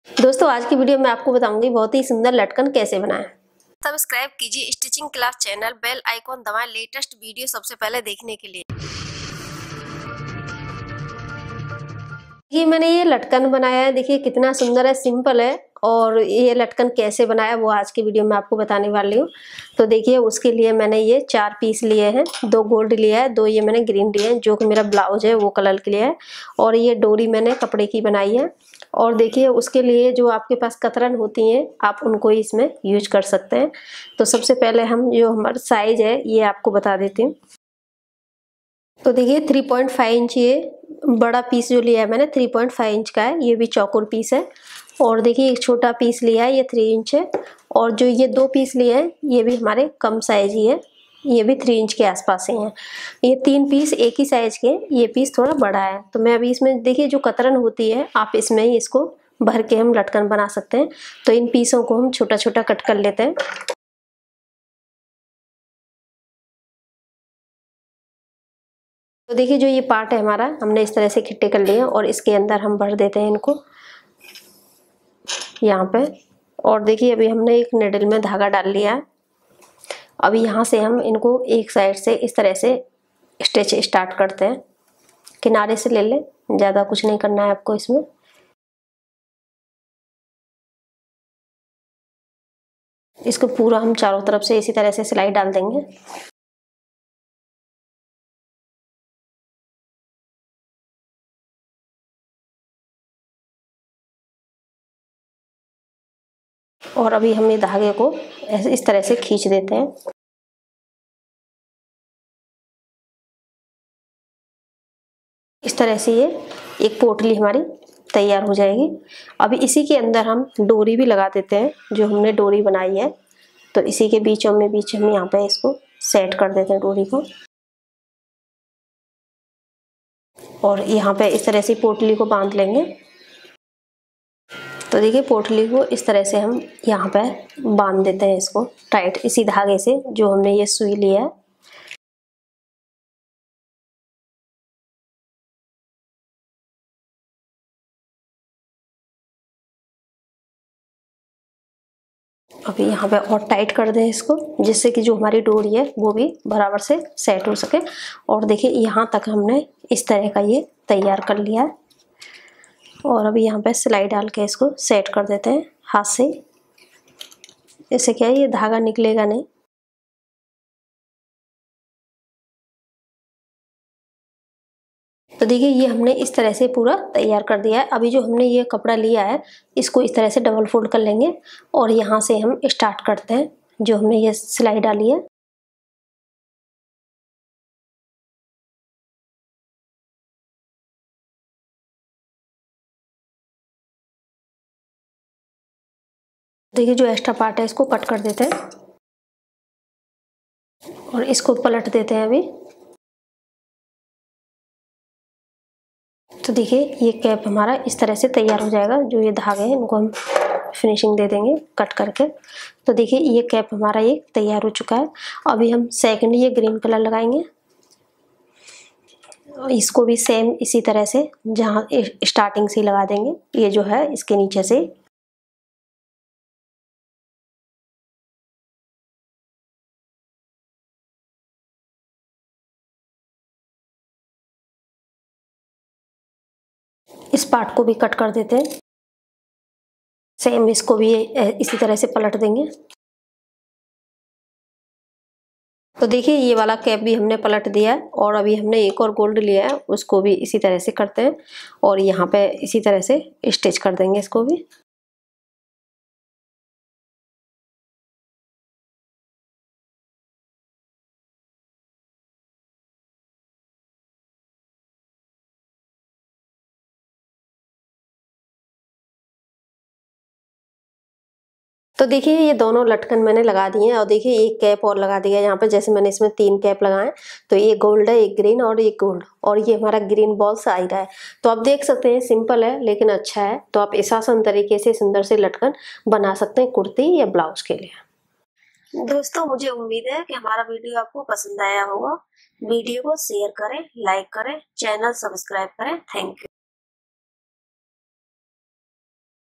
Beautiful Latkan Design, New Model Latkan Design, Cutting And Stitching
Blouse Designs, Latest blouse models, Blouse cutting and stitching, Blouse neck designs

Hi Friends, Today, I am going to show you, how to make stylish blouse back neck design ,in very simple steps. Please watch the video till the end to learn complete method of cutting and stitching. Hope you liked this video, Please share your views & post your comments.

Poonam Gupta
(Stitching Class)